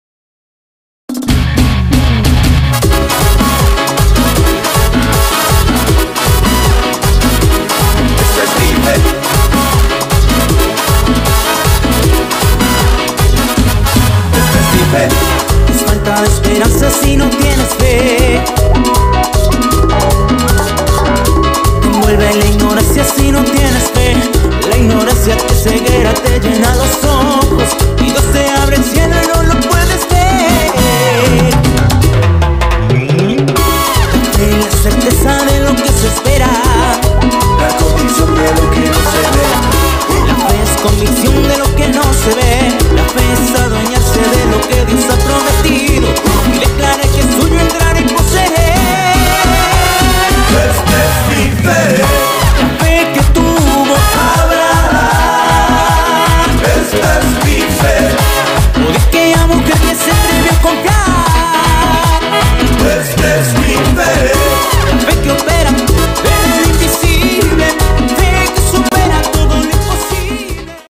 Resumen Mega Recital - jóvenesmedea 💪
Musica cristiana. Contamos con la presencia de diversas bandas en vivo: entre ellas
- Fortaleza.
- Sol de Justicia.
- Rey de reyes.
- Cristo Vence.